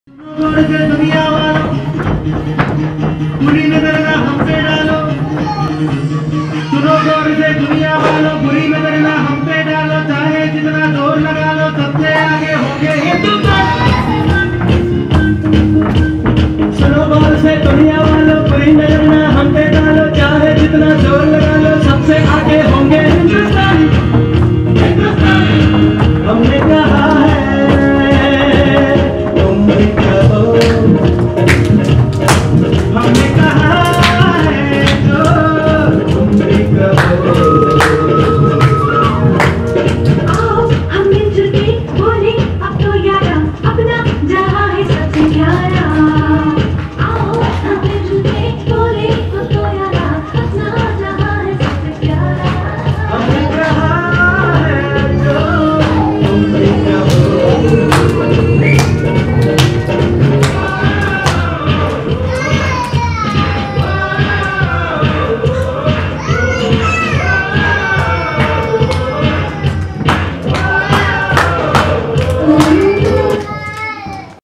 Republic Day Celebrations
As India shines with glory today...Our young ambassadors celebrate with you the pride of being an Indian...Happy Republic Day!